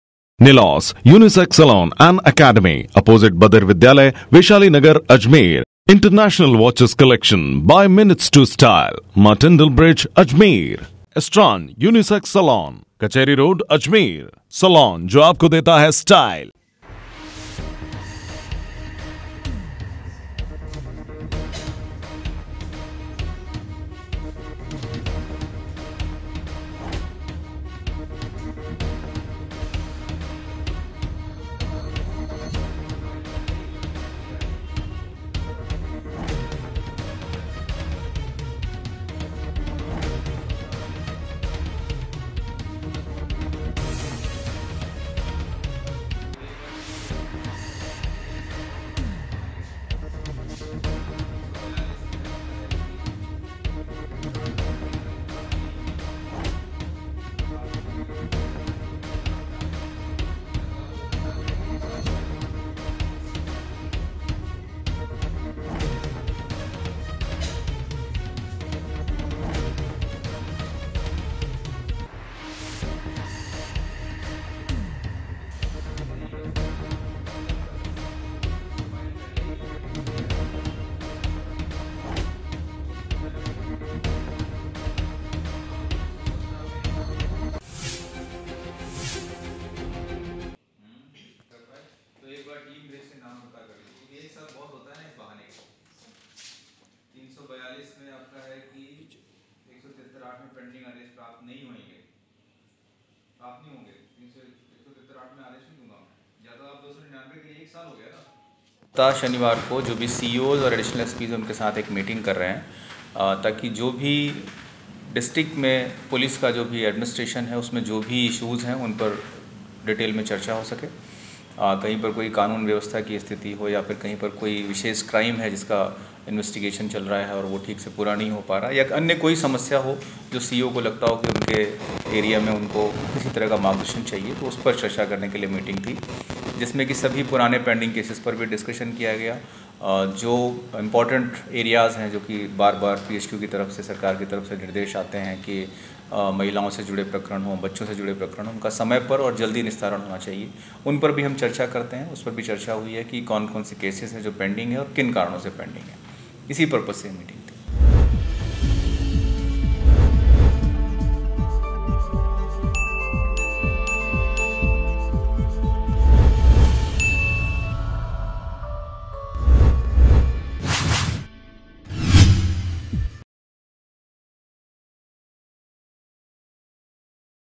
AJMER NEWS | शनिवार को अजमेर एसपी कुंवर राष्ट्रदीप ने ली क्राइम बैठक | MTTV INDIA
शनिवार को अजमेर एसपी कुंवर राष्ट्रदीप ने ली क्राइम बैठक,क्राइम बैठक में सीओ और एडिशनल एसपी रहे मौजूद,अजमेर एसपी ने महिला और बच्चो से जुड़े प्रकरणों का जल्द करें निस्तारण के दिए दिशा निर्देश